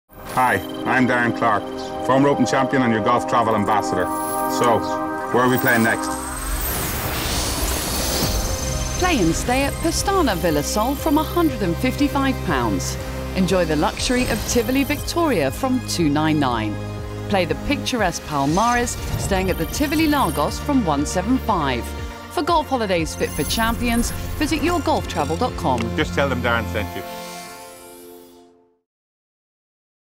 Your Golf Travel Sky Advert feat Darren Clarke, Pestana Vila Sol, Tivoli Victoria & Tivoli Lagos
Your Golf Travel's Sky television advert featuring Your Golf Travel Ambassador, Darren Clarke, and three popular golf holiday promotions. Golf holidays in the Algarve to the popular Pestana Vila Sol, five-star Tivoli Victoria and one of the best in the West Algarve Tivoli Lagos are just three offers from the world's largest golf travel company.